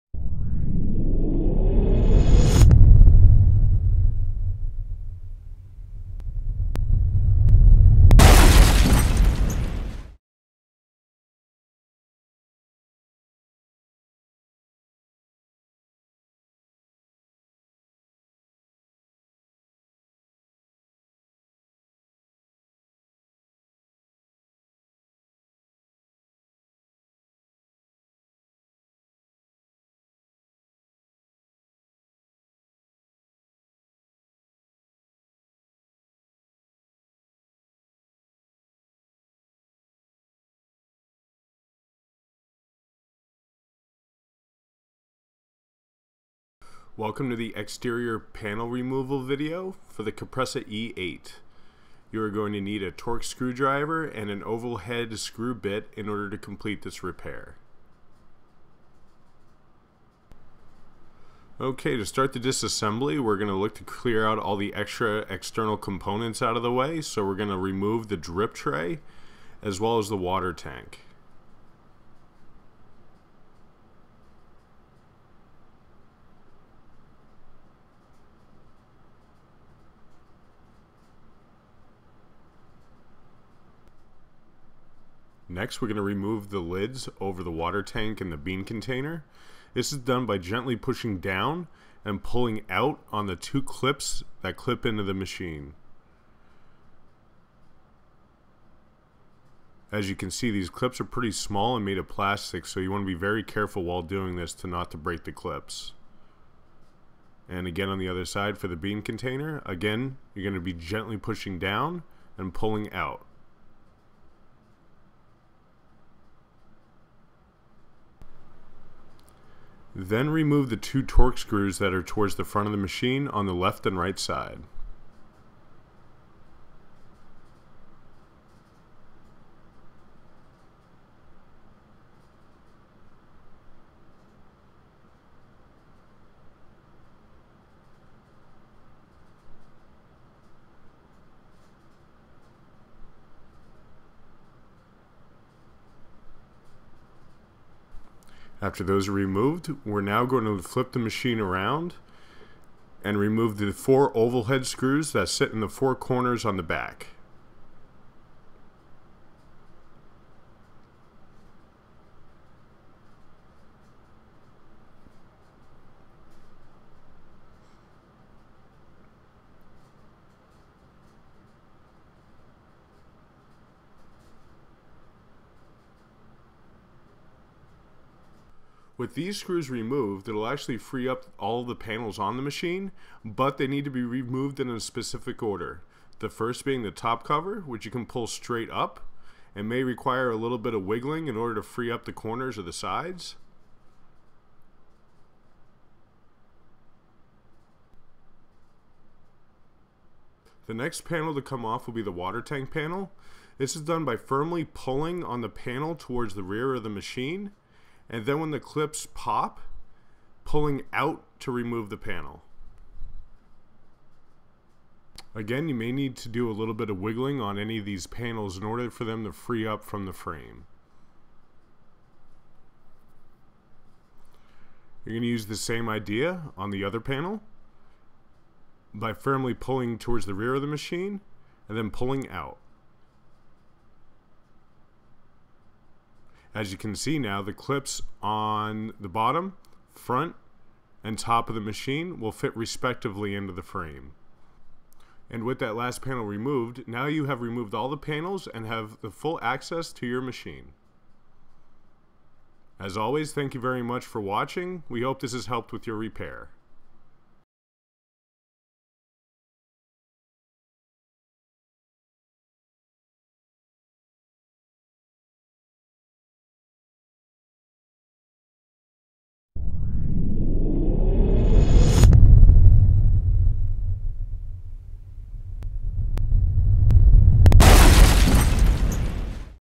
How to Open Jura Capresso-Impressa C-E-F Coffee Machines
This video details the step-by-step process and tools required to open Jura C5-C9-C60-C90-E8-E9-F7-F8-F9 coffee machines. The model shown in the video is an Impressa E8; however, the screw placement and process to open the machine are the same for the other models listed.